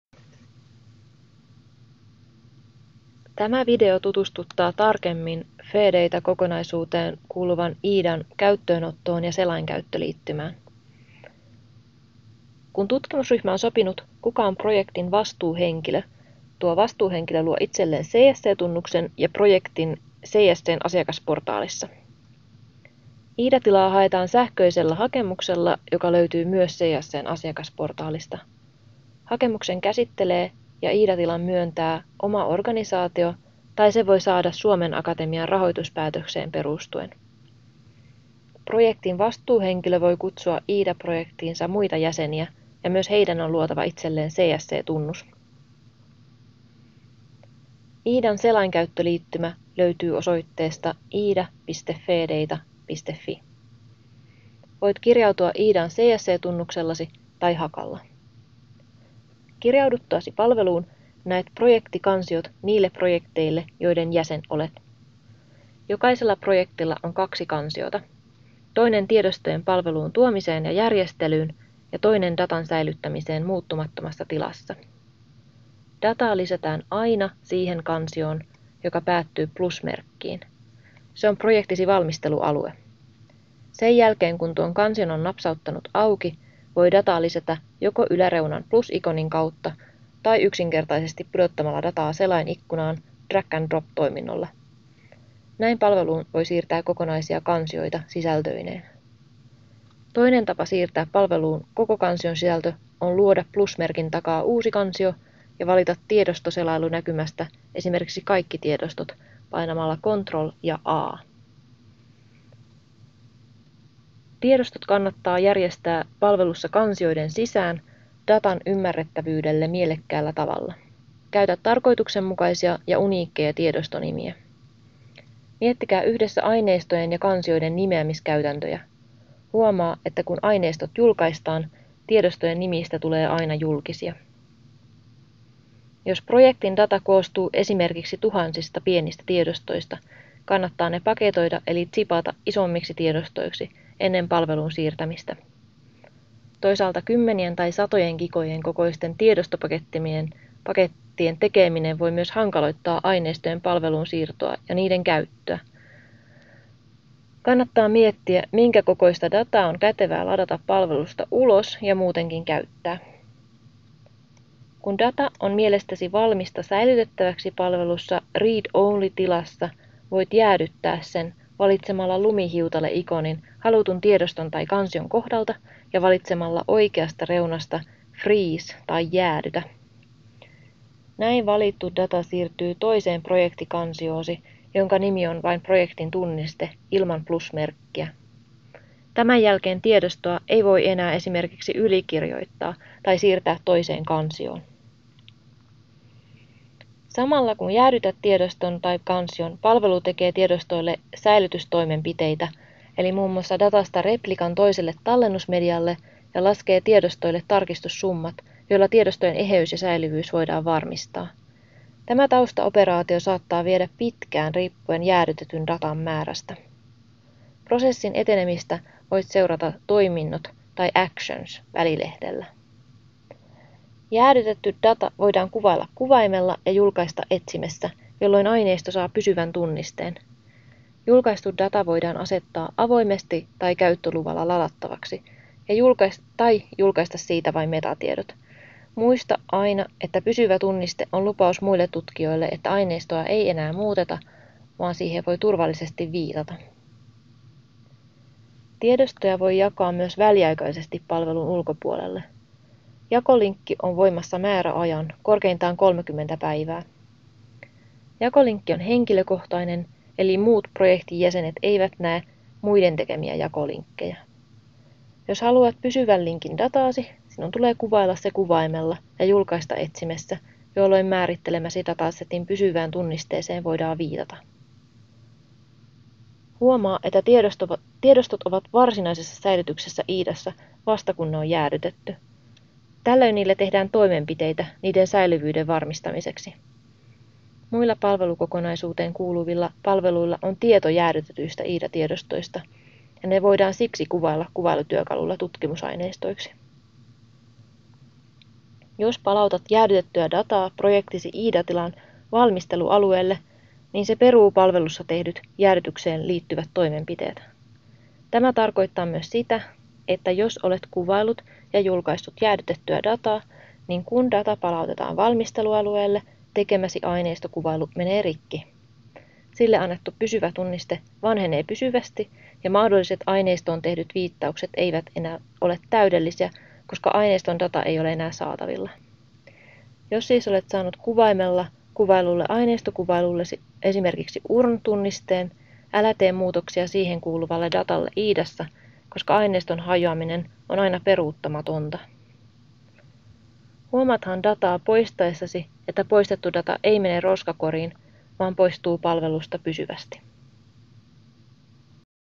Tutkimusdatan säilytyspalvelu IDAn esittely (2/2)
IDA-esittelyn toinen osa käy läpi palvelun toiminnallisuudet sen selainkäyttöliittymässä.